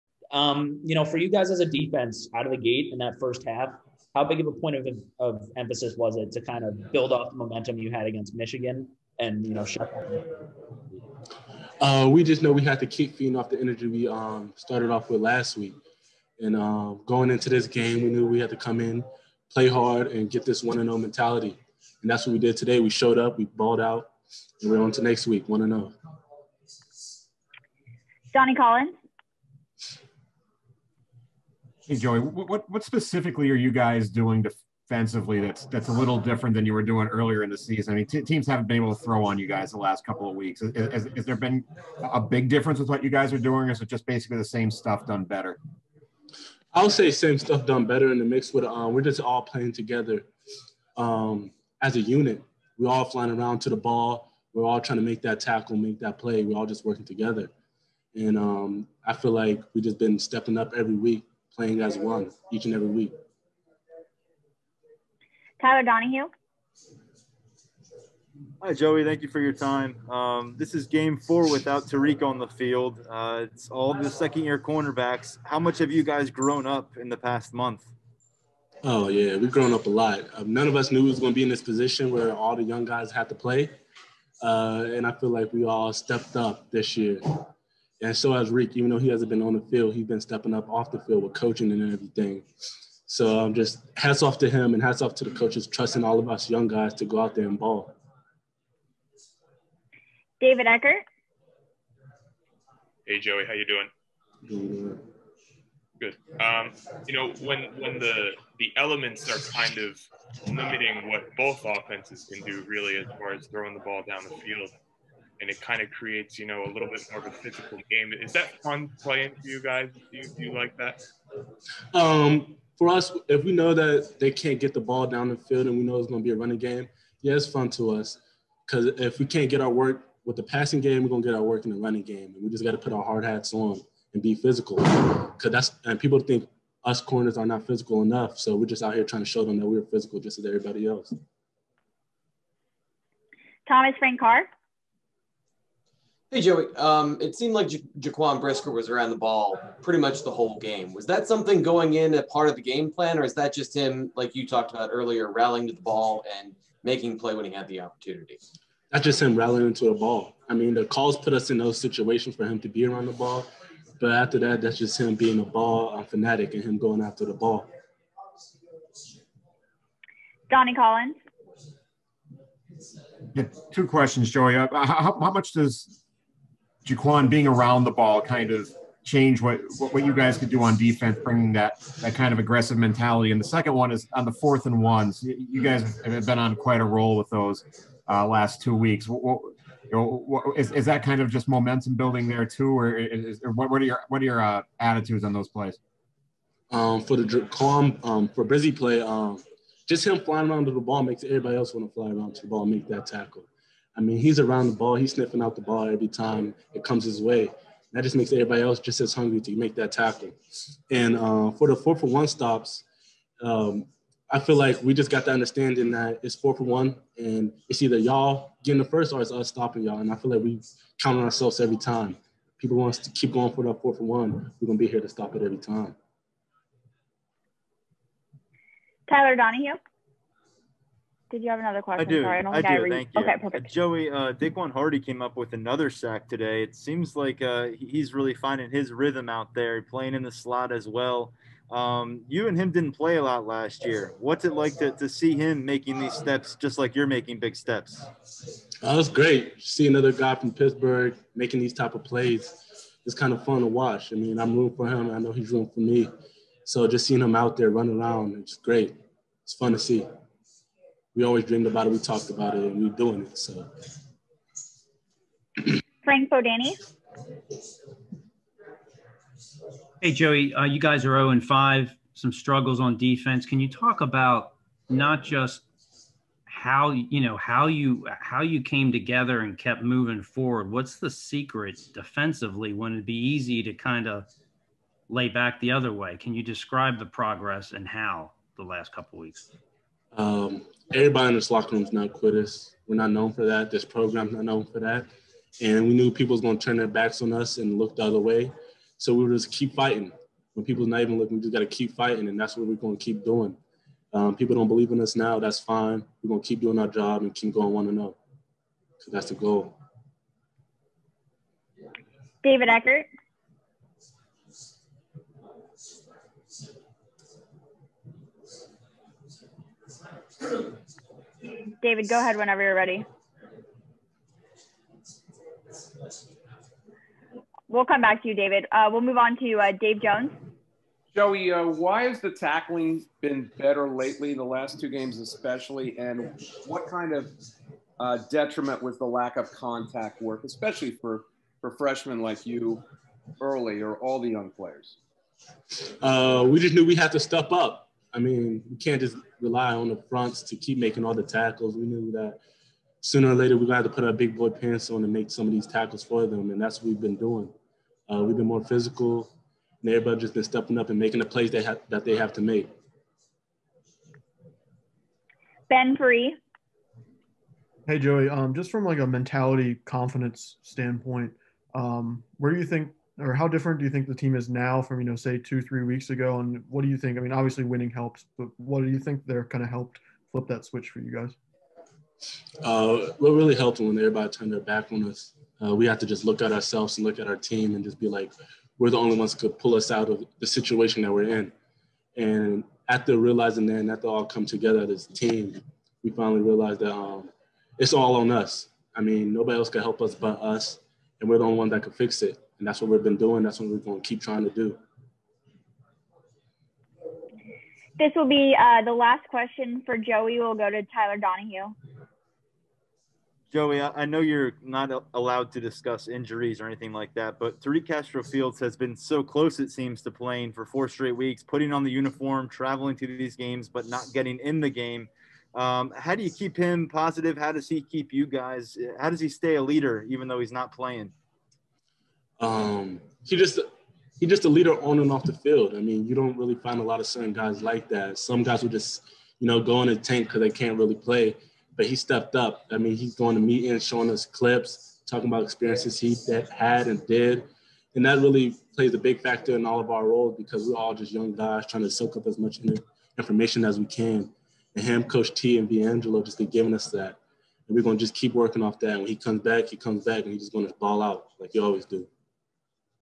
PSU CB Joey Porter Jr. Discusses Win Over Rutgers | NSN
Porter talks about the Lions' 23-7 win.